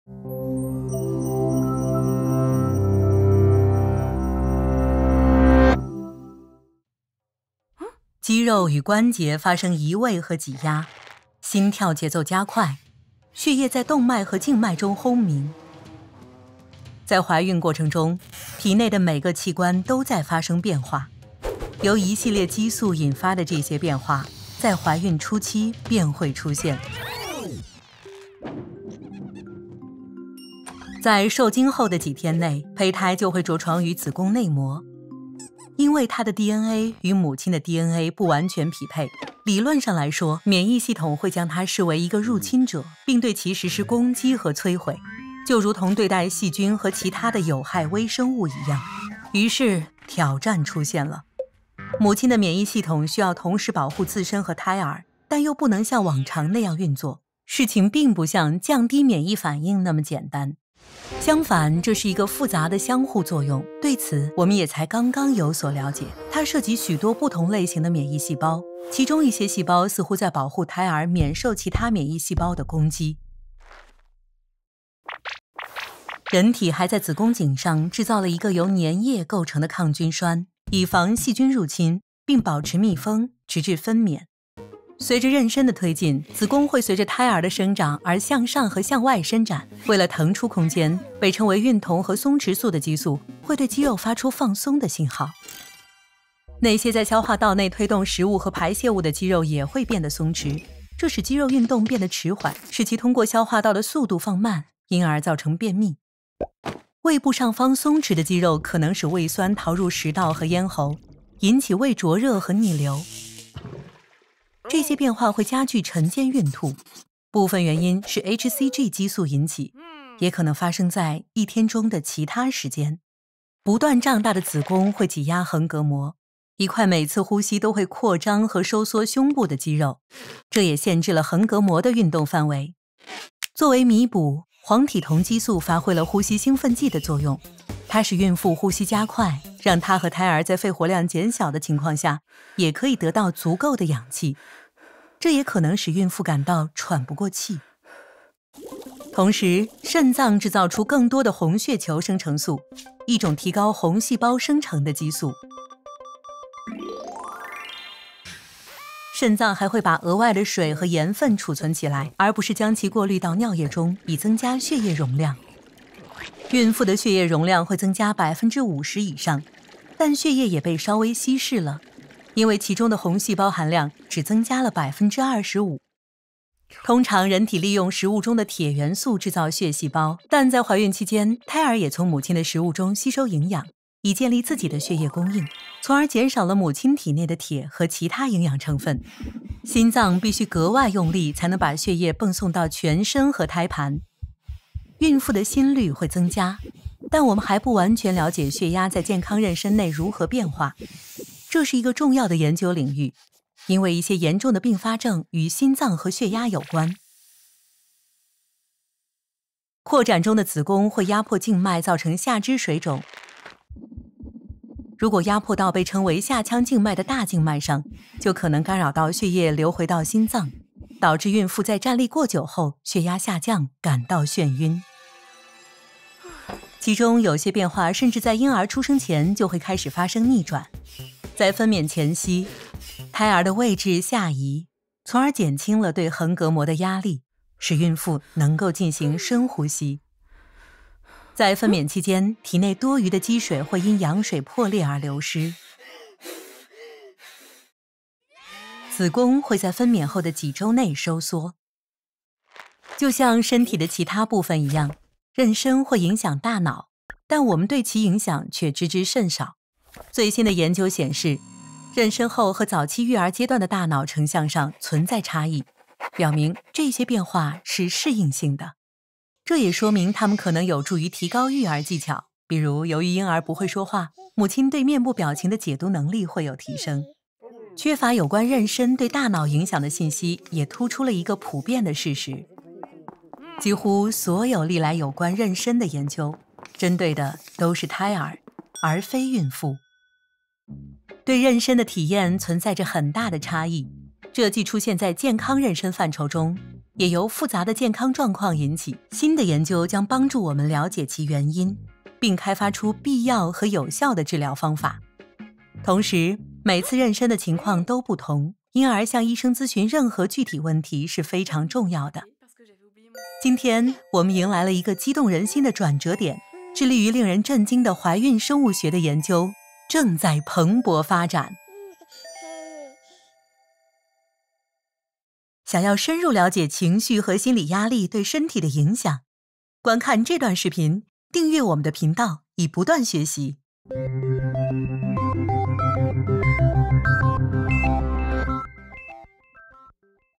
怀孕的惊人影响
肌肉和关节运动和碰撞。心跳加快。血液在动脉和静脉中咆哮。怀孕期间，身体的每个器官都在发生变化。这些变化是由怀孕初期开始的一系列激素引发的。探索我们所知道的和不知道的怀孕对身体和大脑的影响。

课程老师 TED-Ed，导演 Charlotte Cambon, Roxane Campoy。